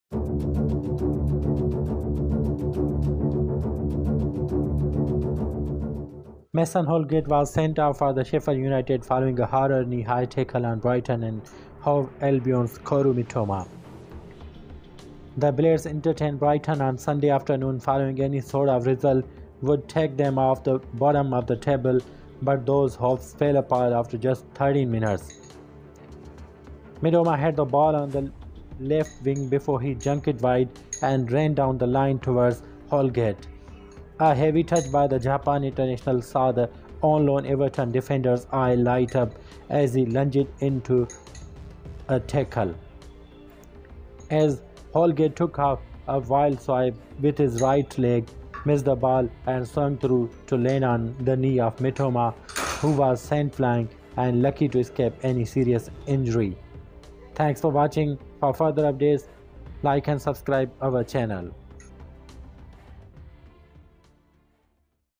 Mason Holgate sent off for ‘RECKLESS LEG_BREAKING’ tackle against Brighton - Mason Holgate Red Card
Mason Holgate sent off for ‘reckless leg-breaking’ tackle against Brighton
Sheffield United vs Brighton
Mason Holgate Red Card